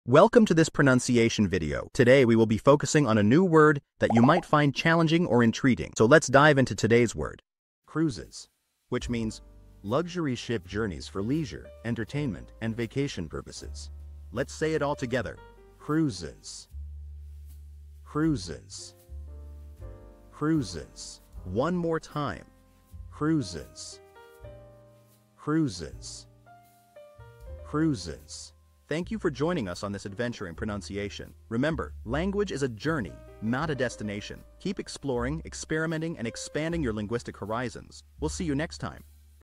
How to pronounce Cruises
Master the Pronunciation of 'Cruises' - which means : Luxury ship journeys for leisure, entertainment, and vacation purposes. 📝 with @PPrononciation 🎓🗣️ - [Your Guide to English]

Learn to pronounce 'Cruises' perfectly in English with our easy and engaging tutorial by @PPrononciation! 📖📽️ In this comprehensive video, we provide you with native speaker examples 🗣️👂 and phonetic breakdowns 🎵 to ensure you get the pronunciation just right.

How to say Cruises in English